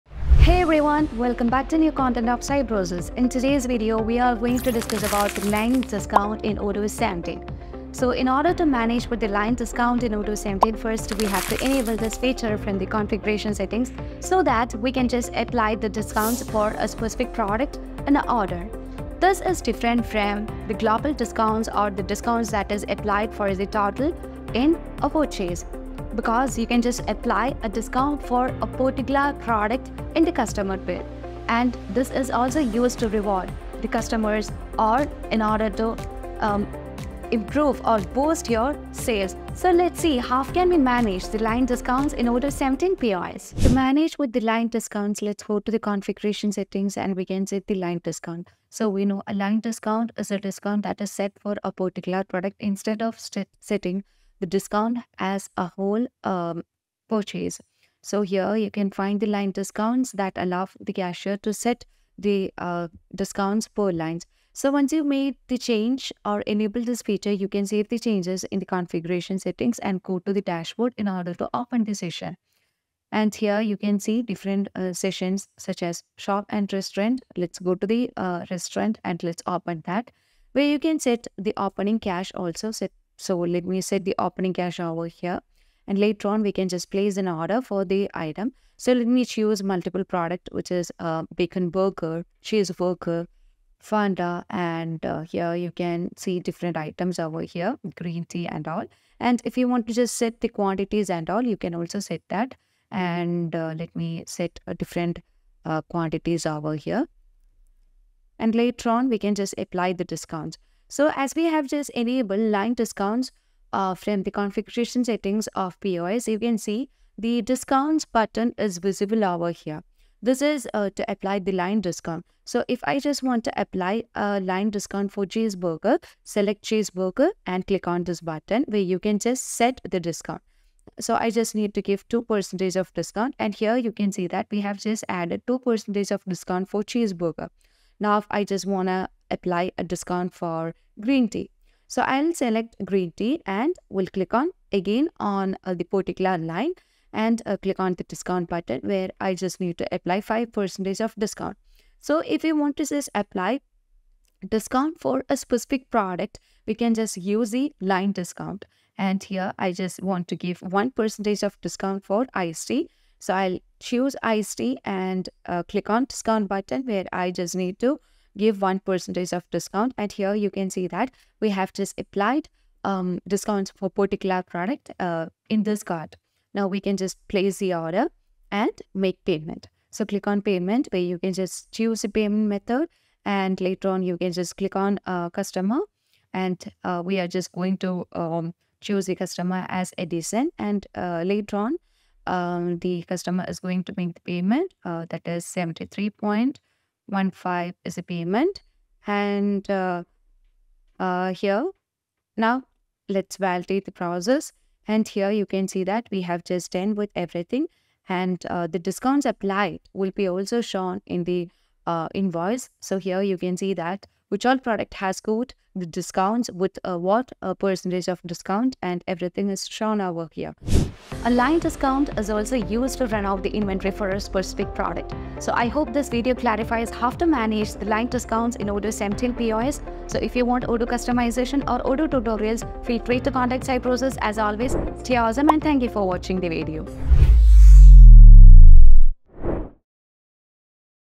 How to Manage Line Discount in Odoo 17 POS | Odoo 17 Functional Tutorials
A line discount in a POS system refers to a reduction in the price of a specific item on a customer's bill. It's different from a total order discount, which applies to the entire purchase.

Line discounts offer several advantages:

Promotional flexibility: You can provide discounts on particular products to clear out inventory, run targeted promotions, or reward specific customers.
Increased sales: Discounts can incentivize customers to buy more by making products more affordable.
Improved margins: You can use line discounts strategically to maintain profitability even with reduced prices.
There are two main types of line discounts:

Percentage discount: This reduces the price of an item by a specific percentage.
Fixed amount discount: This reduces the price of an item by a set dollar amount. For example, you could offer a $5 discount on a particular toy.
Here's how line discounts typically work in a POS system:

Adding the item: The cashier scans the item or enters the product code into the POS.
Applying the discount: The POS system might automatically apply a pre-programmed line discount to certain items. Alternatively, the cashier may have the option to manually enter a discount for a specific item.
Calculating the final price: The POS system calculates the discounted price by subtracting the discount amount from the original price of the item.
Receipt reflection: The final price with the applied discount is reflected on the customer's receipt.
Line discounts are a powerful tool for POS systems, enabling you to offer targeted promotions and manage your pricing strategy effectively.